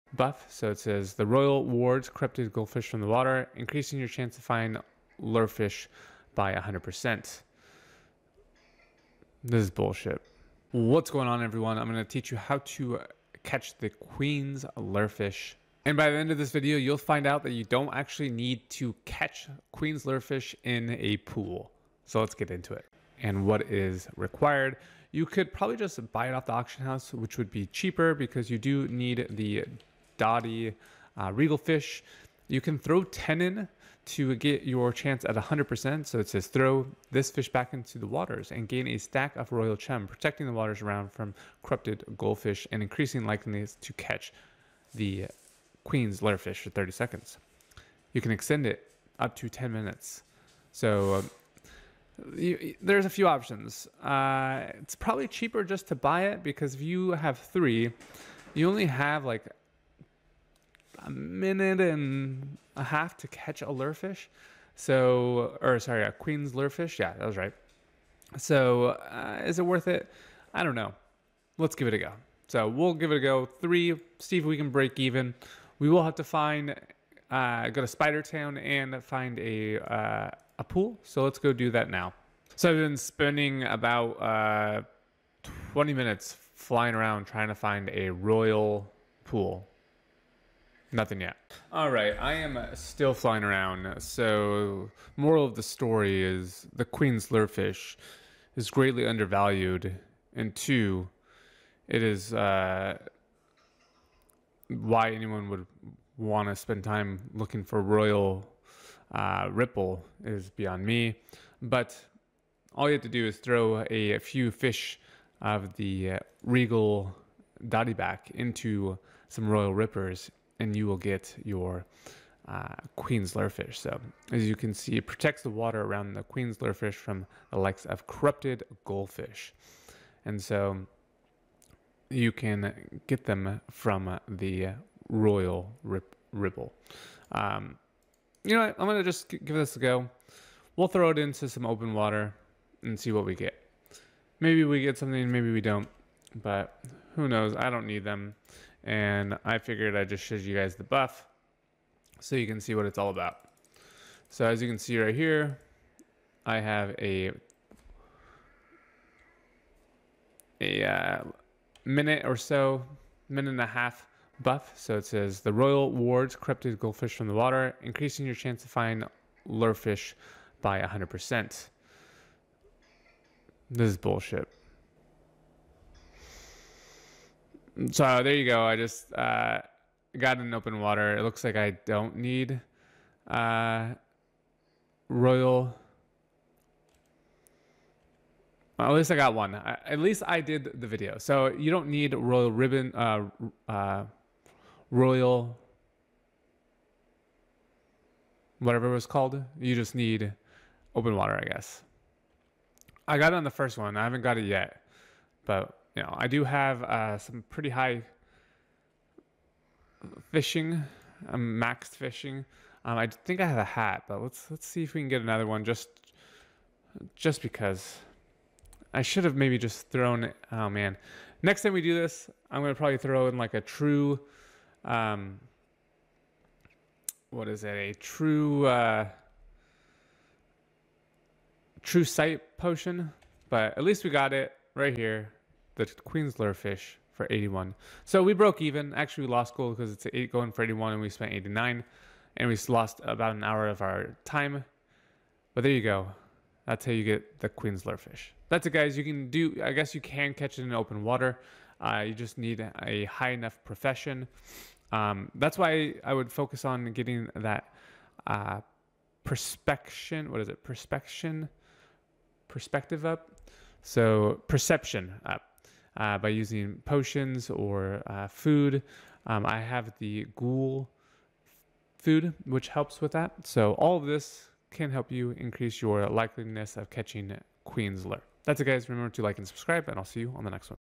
How To Fish Queen's Lurefish Without Royal Ripple Pools
Are you struggling to catch the elusive Queen's Lurefish but can't find any Royal Ripple Pools? Don't worry, in this video, I’m going to show you how you can still catch this rare fish without needing those special pools! Whether you're just looking to complete your collection or aiming for that high-level quest, these techniques will help you reel in the Queen's Lurefish in no time.

📋 What You’ll Learn In This Video:

The exact locations where you can find Queen's Lurefish without using Royal Ripple Pools.
The best bait and gear for maximizing your chances.
Advanced fishing strategies to increase your catch rate.
How to fish efficiently without wasting time.
Make sure you watch till the end to see my secret spot!

🕒 Timestamp: 0:00 Intro
0:30 Queen’s Lurefish Overview
1:00 Alternative Fishing Spots Without Royal Ripple Pools
2:30 Best Gear and Bait Setup
3:30 Advanced Fishing Tips and Techniques
5:00 Secret Fishing Spot Revealed!
6:00 Closing Thoughts